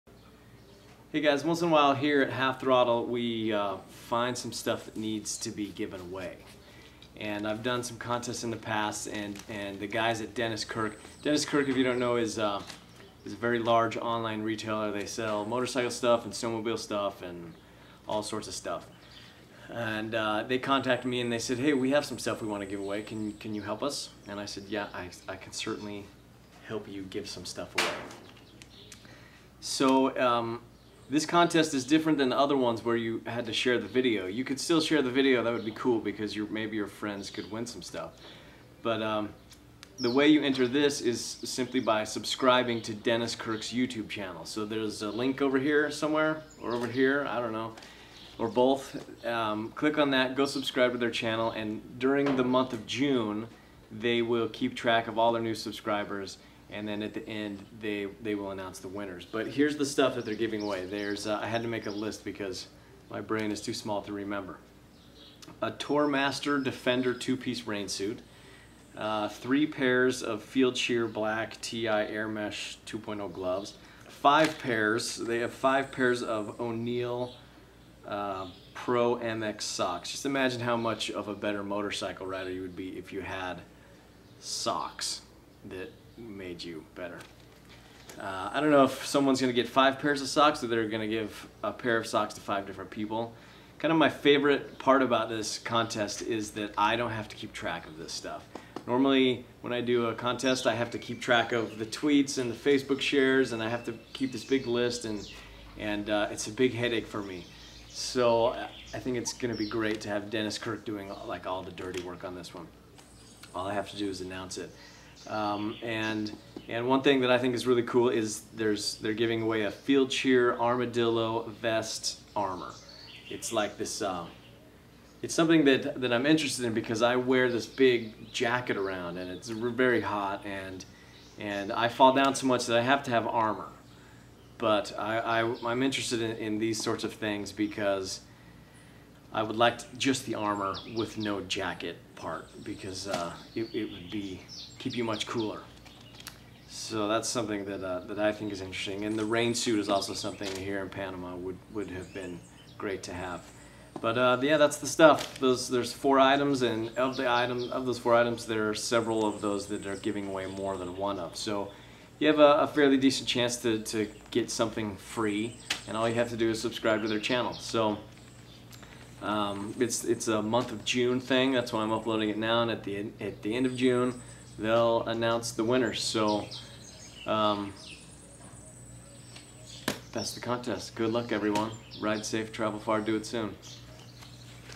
Dennis Kirk free stuff
It's become a sort of public service here at HalfThrottle.  Dennis Kirk contacted me with some goodies they want to hand out in exchange for more subscribers to their YouTube channel.

Here are the links to the stuff being given away if you want to have a closer look: